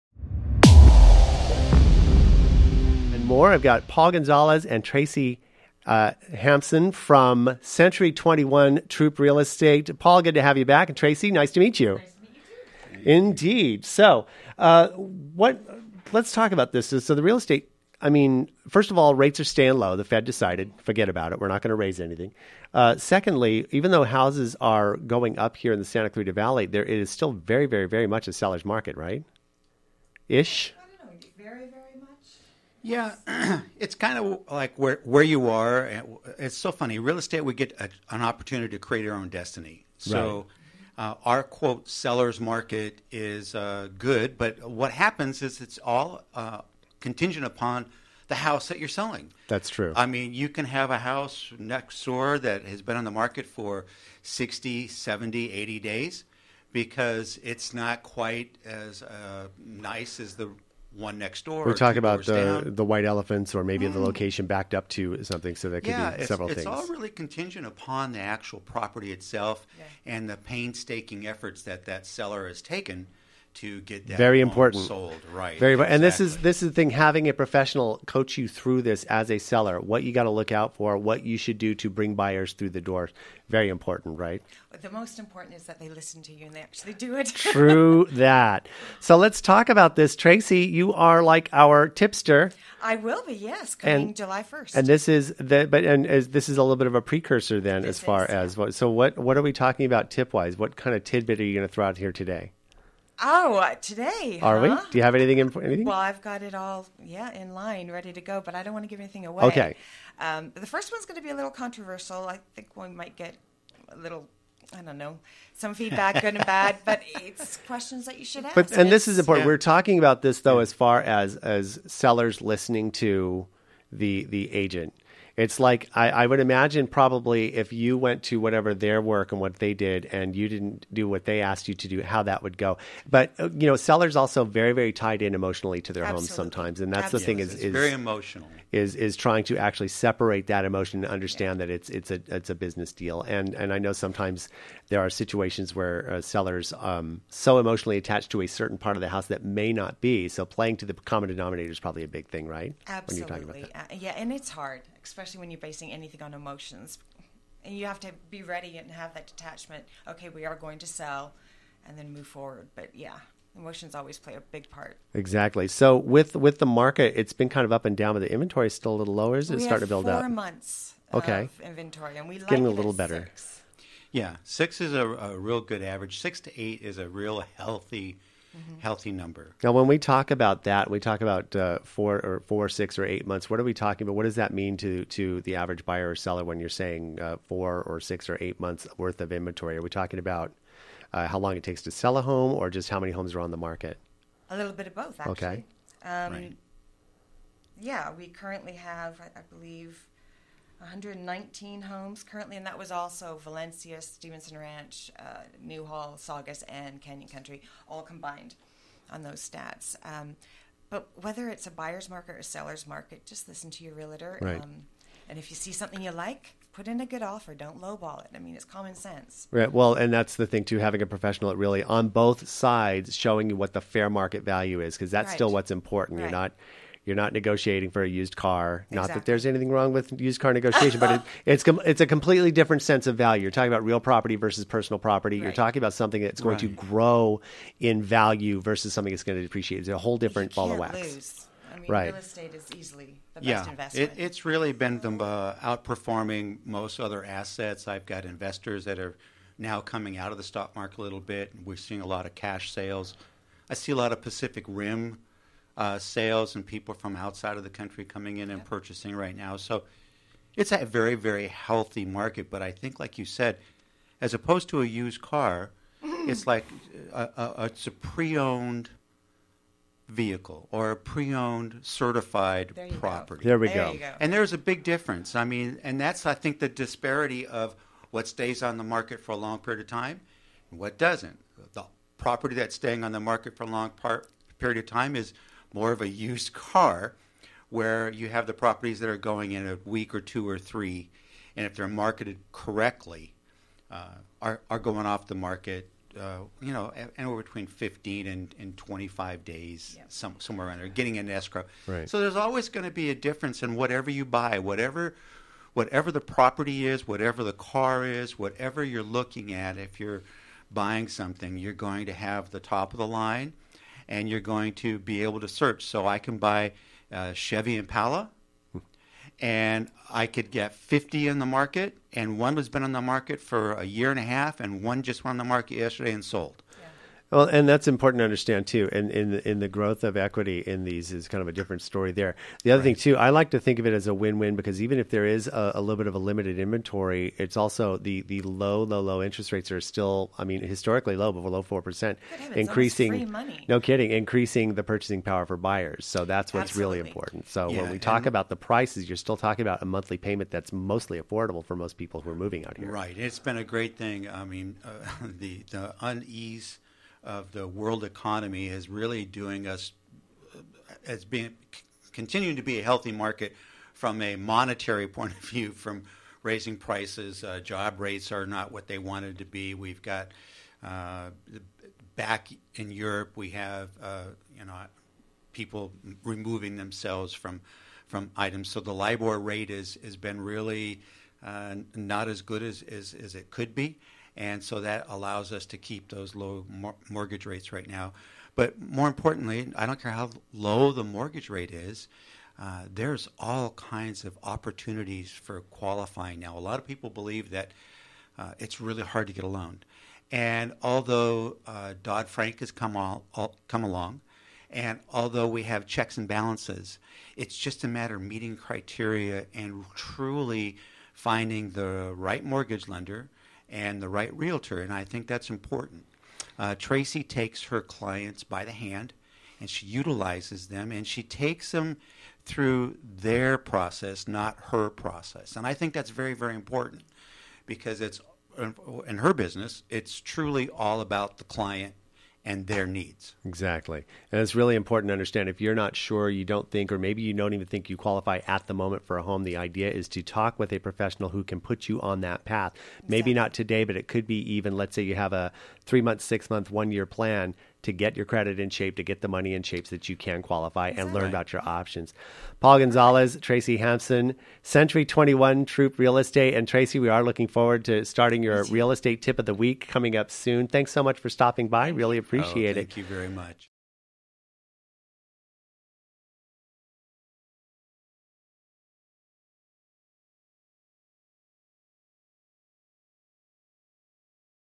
Tracey Hampson From Century 21 Troop Real Estate - June 16, 2016 - KHTS - Santa Clarita
After building a successful real estate business in residential sales, Tracey joined forces with Troop Real Estate, Inc. in Santa Clarita in 2011. She assembled a dedicated team of service providers, which allows her to focus on what she does best, navigating and managing real estate transactions for her clients. Tracey is recognized as one of the top Realtors for gross commission and sales volume in Troop Real Estate, Inc.  She was also acknowledged by her peers as “Agent of the Year’ in 2013 and ""Top Listing Agent in 2014."" Her Valencia headquarters serves the many communities of the region, specializing in luxury and high end real estate.